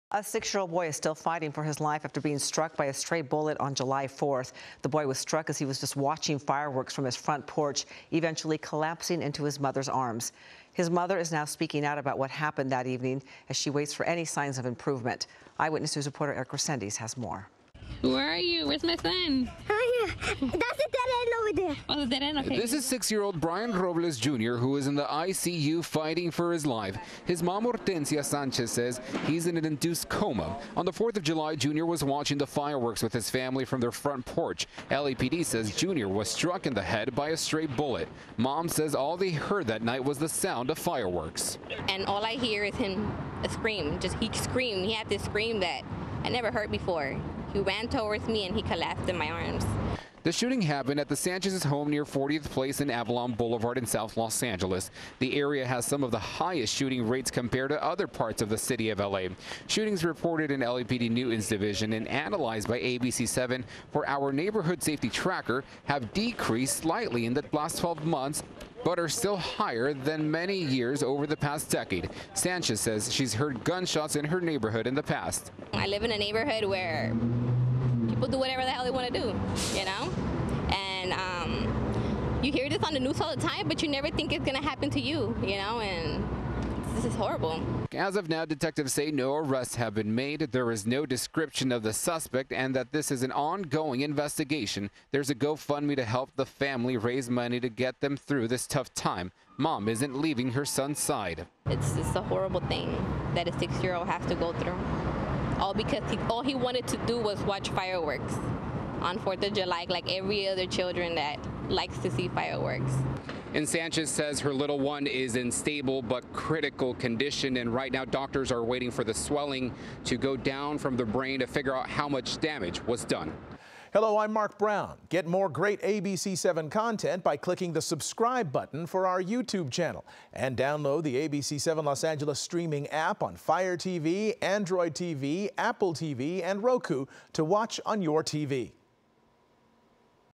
Mother recalls moment son was hit by stray bullet on 4th of July
The mother of a young boy who's fighting for his life after being hit by a stray bullet in South Los Angeles on the 4th of July recalled the terrifying moment she knew her family's celebration would be cut short.